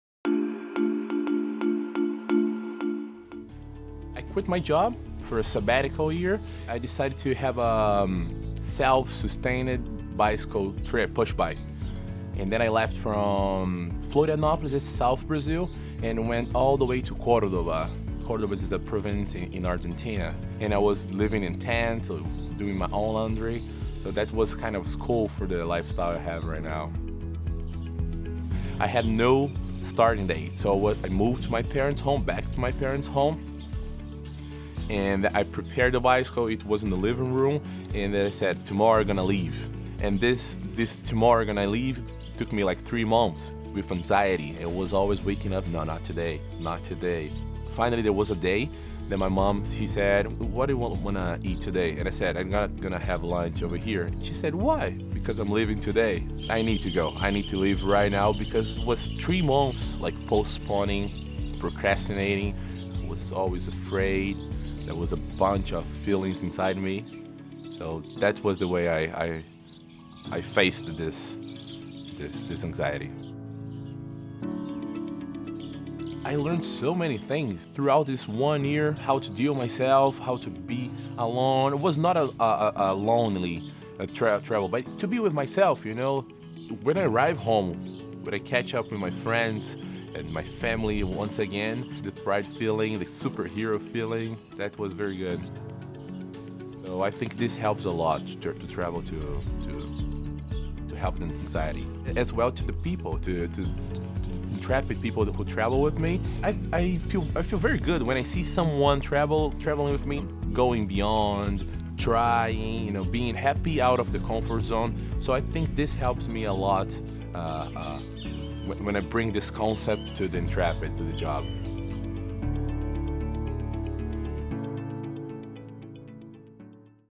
Intrepid Leader Pedro
Heading off on an adventure can be a daunting experience. Pedro shares his story of taking the leap to travel on his own, getting out of his comfort zone, and learning how to be alone.